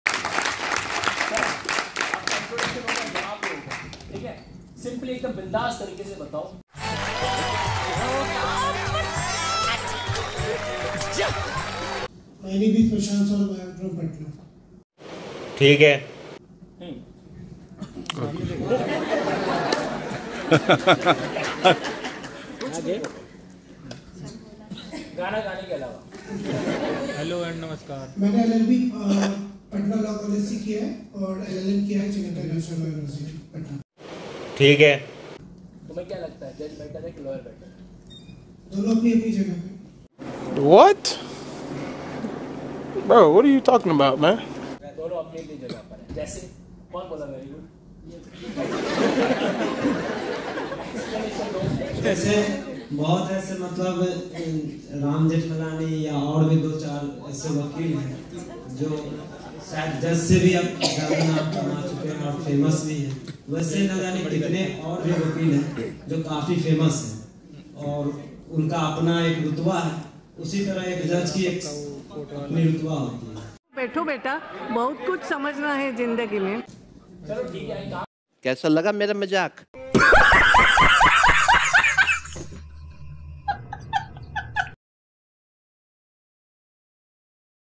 Judiciary or Advocacy Raipur Event Part 2 Fun Moment with Linking Law
Judiciary Or Advocacy ?
Judiciary or Advocacy Raipur Event Part 2 Video
Fun Moment with Linking Law
judiciary best educator Tansukh Paliwal Sir